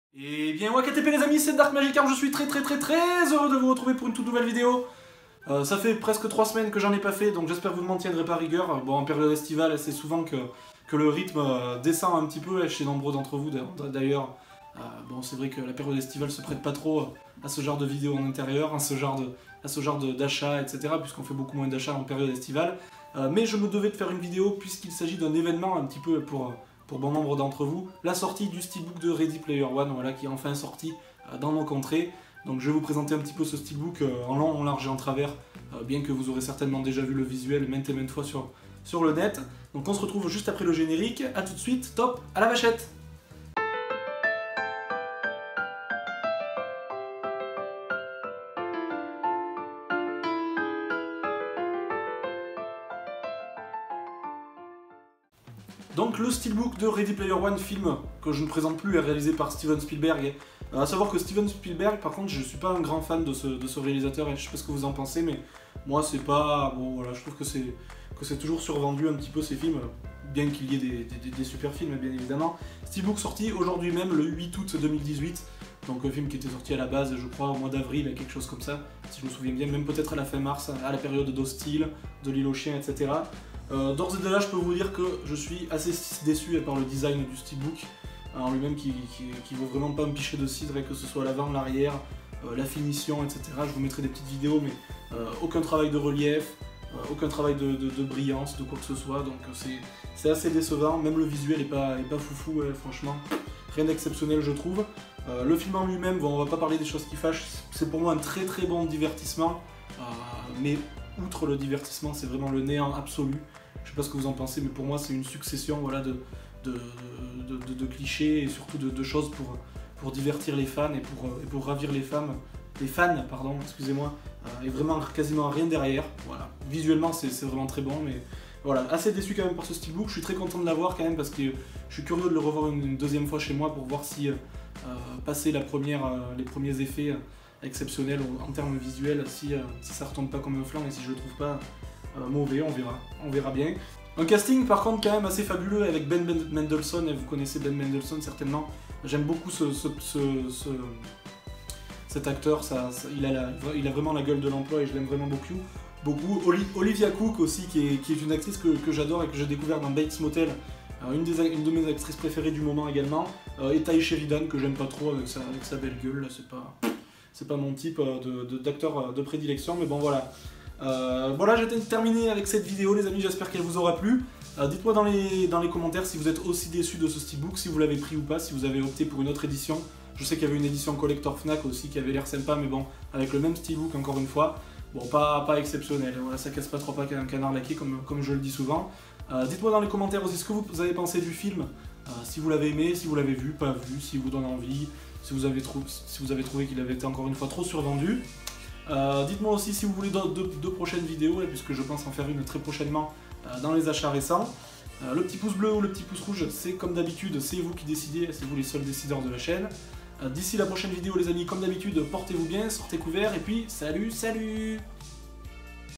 UNBOXING STEELBOOK READY PLAYER ONE 4K UHD EDITION FNAC
Wakatépé les amis ! On se retrouve en cette période estivale pour un des évènements de l'année pour tous les aficionados de science-fiction et de pop culture : la sortie Française du steelbook de Ready Player One !